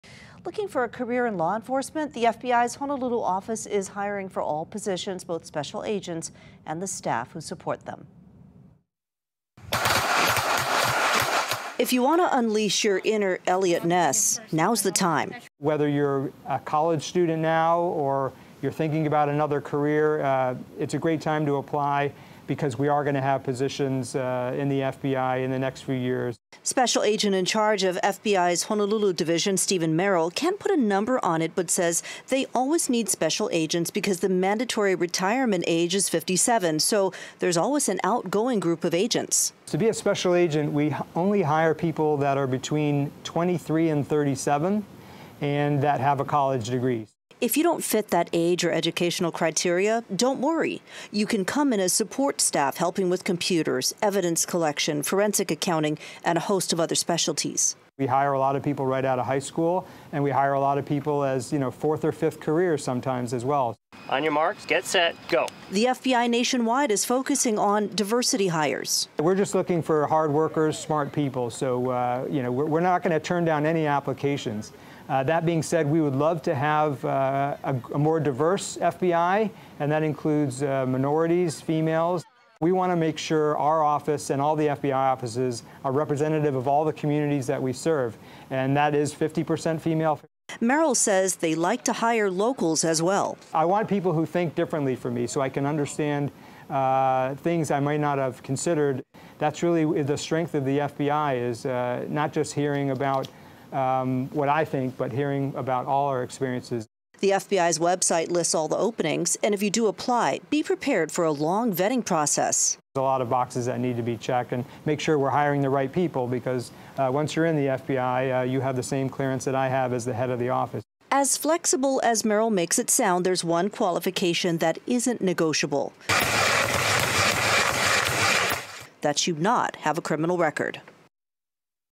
FBI Honolulu recruiting for all positions
Looking for a career in law enforcement? The FBI's Honolulu office is hiring for all positions- both special agents and the staff who support them.